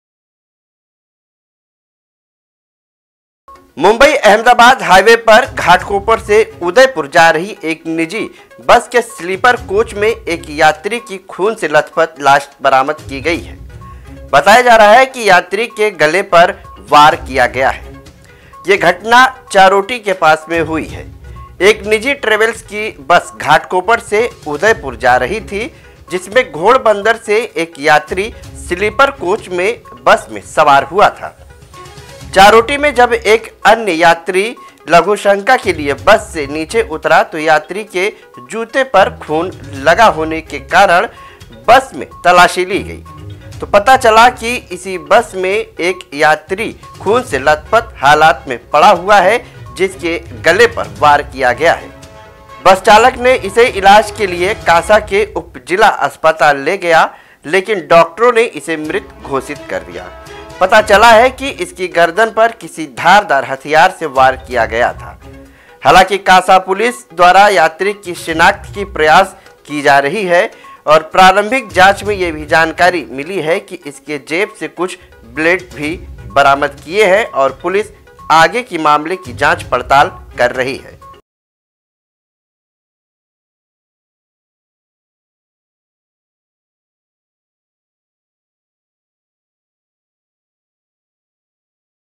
मुम्बई से उदयपुर जानेवाली बस में यात्री के गले पर वार,हुई मौत || Islamuddin Khan|| Prahar Times
मुम्बई से उदयपुर जानेवाली बस में यात्री के गले पर वार,हुई मौत || Islamuddin Khan